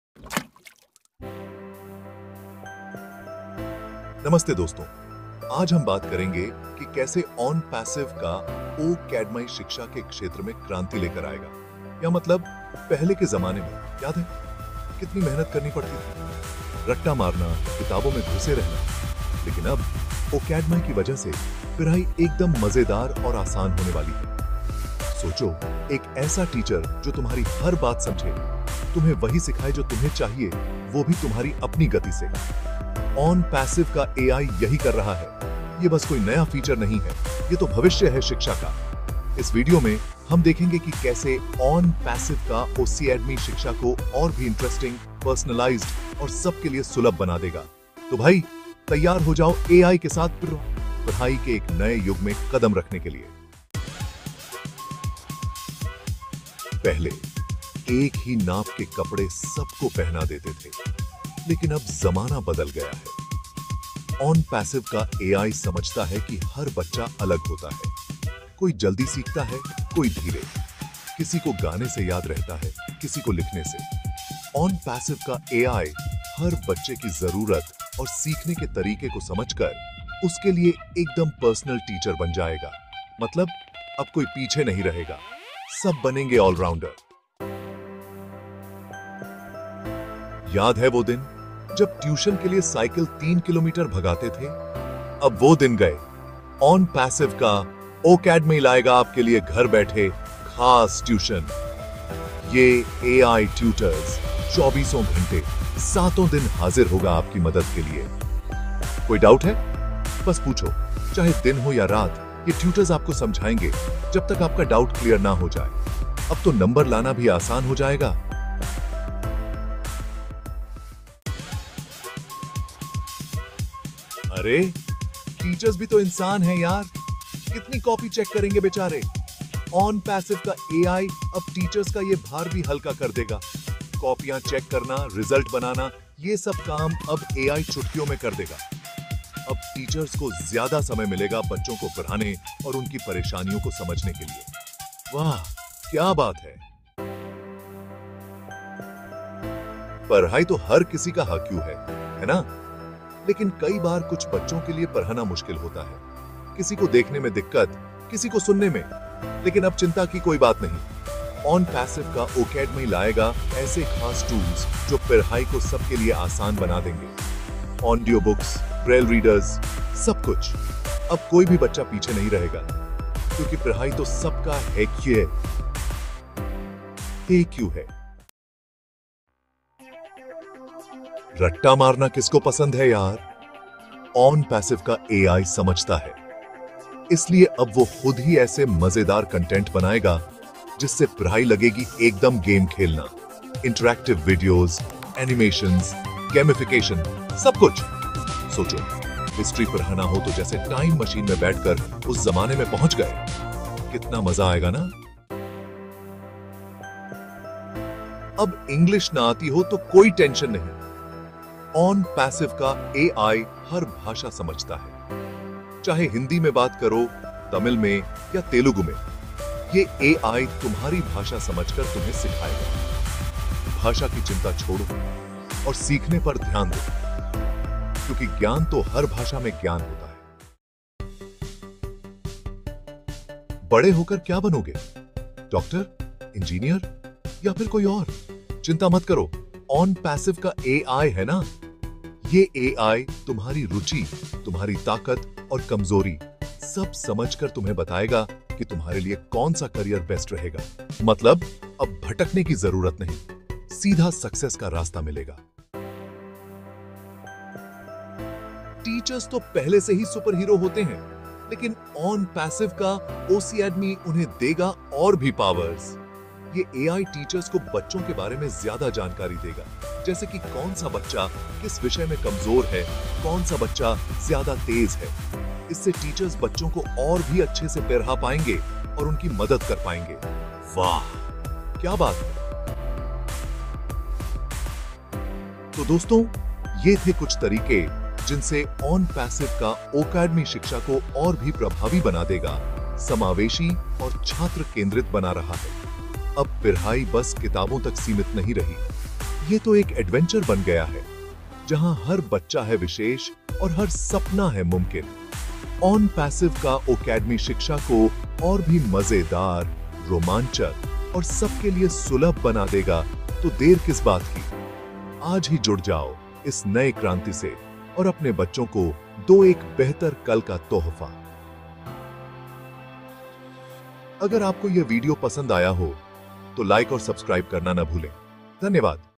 ONPASSIVE || O Cademy ...Smart Education with AI . शिक्षा के क्षेत्र में क्रांति लाएगा ..
ONPASSIVE का AI शिक्षा में क्रांति ला रहा है! यह वीडियो दिखाएगा कि कैसे ONPASSIVE का AI शिक्षा को मजेदार और आसान बनाता है, छात्रों की जरूरतों को समझ कर एक पर्सनलाइज्ड और इंटरेस्टिंग लर्निंग अनुभव प्रदान करता है। जानिए कैसे AI ट्यूटर्स 24/7 उपलब्ध होते हैं, कैसे वे बच्चों को उनके दृष्टिगत और श्रवण चुनौतियों के बावजूद सहायता करते हैं, और कैसे AI टूल्स जैसे ऑडियोबुक्स और ब्रेल रीडर्स शिक्षा को सभी के लिए सुलभ बनाते हैं। देखिए कैसे AI कंटेंट को इंटरैक्टिव और मजेदार बनाता है, और कैसे यह टीचर्स के बर्डन को कम करके उन्हें बच्चों की व्यक्तिगत जरूरतों पर ध्यान केंद्रित करने में मदद करता है।